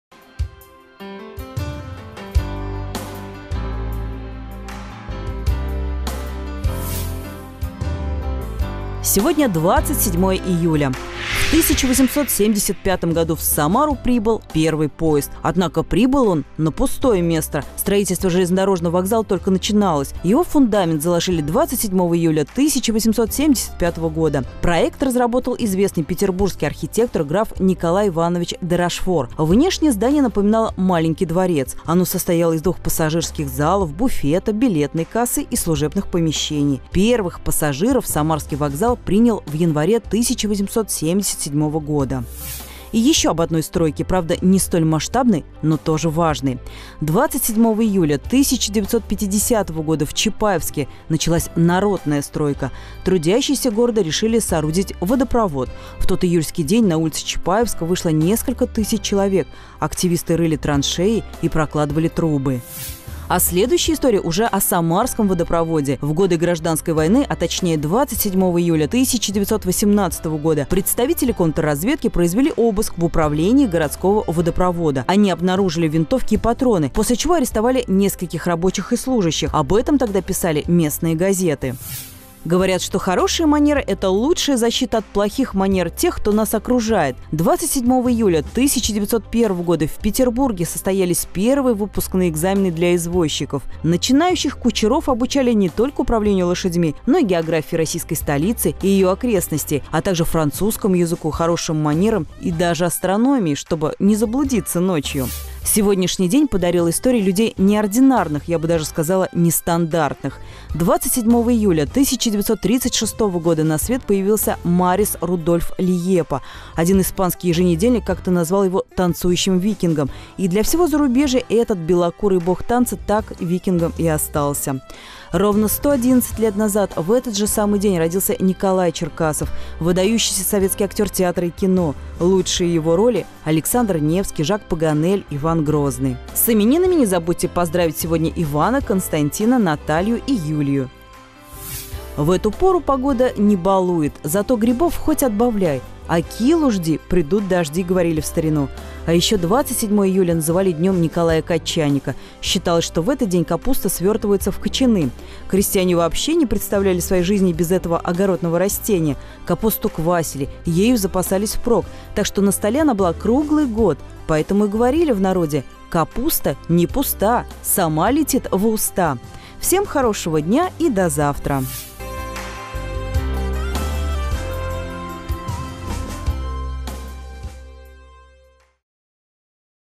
Календарь Губернии 27.07.2014
В 1875-м году в Самару прибыл первый поезд. Однако прибыл он на пустое место. Строительство железнодорожного вокзала только начиналось. Его фундамент заложили 27 июля 1875 года. Проект разработал известный петербургский архитектор, граф Николай Иванович де Рошфор. Внешне здание напоминало маленький дворец. Оно состояло из 2-х пассажирских залов, буфета, билетной кассы и служебных помещений. Первых пассажиров самарский вокзал принял в январе 1877 года.

И ещё об одной стройке – не менее важной и масштабной. 27 июля 1950 года в Чапаевске началась «народная стройка». Трудящиеся города решили соорудить водопровод. В тот июльский день на улицы Чапаевска вышло несколько тысяч человек. Активисты рыли траншеи и прокладывали трубы.

А следующая история уже о самарском водопроводе. В годы Гражданской войны, а точнее, 27 июля 1918 года, представители контрразведки произвели обыск в управлении городского водопровода. Они обнаружили винтовки и патроны, после чего арестовали нескольких рабочих и служащих. Об этом тогда писали местные газеты.

Говорят, что «хорошие манеры – это лучшая защита от плохих манер тех, кто нас окружает». 27 июля 1901 года в Петербурге состоялись первые выпускные экзамены для извозчиков. Начинающих кучеров обучали не только управлению лошадьми, но и географии российской столицы и её окрестностей, а также французскому языку, хорошим манерам и даже астрономии, чтобы не заблудиться ночью.